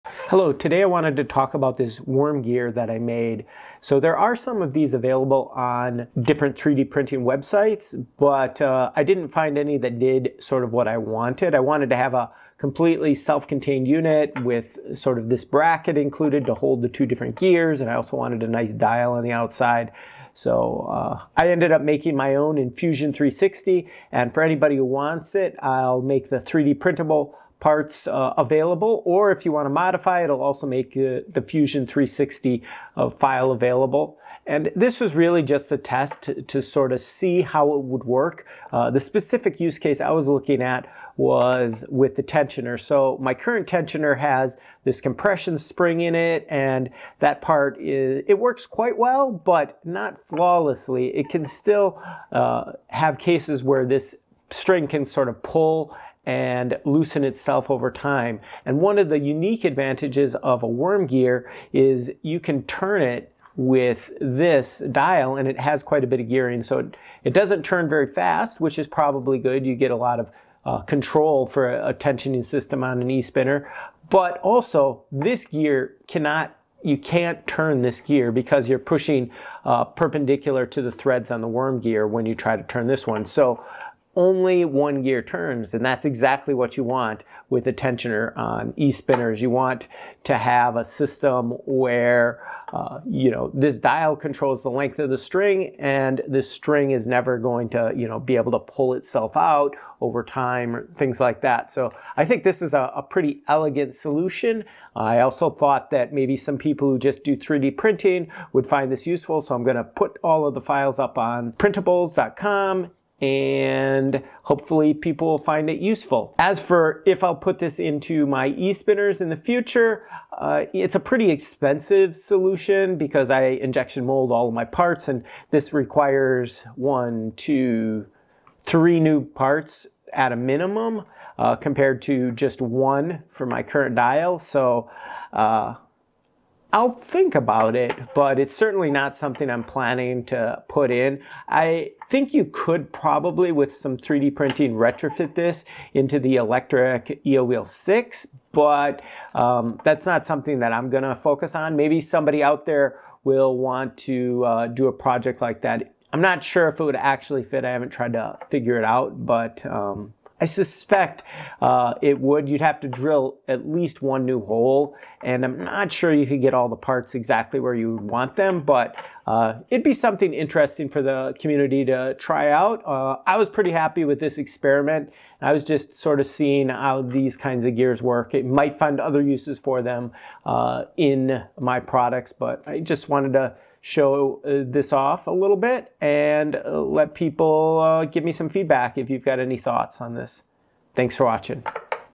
Spin 158 - Worm Gear Tensioner Test
I created this video as a test to see how a work gear works.  The stl and fusion 360 files for this project are available